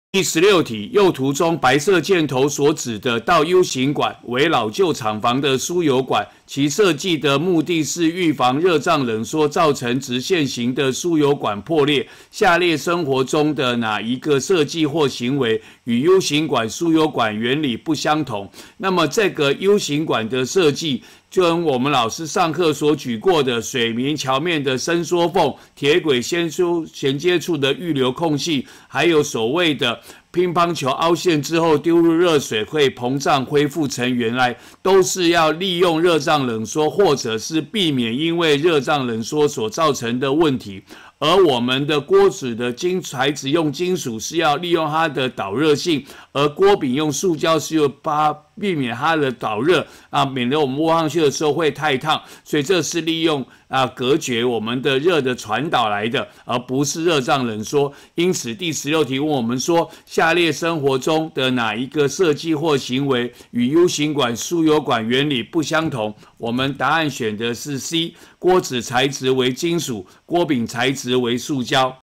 109學年上8年級理化三段第16題
「倒U形管」設計的目的是預防熱脹冷縮造成直線形的輸油管破裂。生活中的哪一個設計或行為，與U形輸油管原理不相同？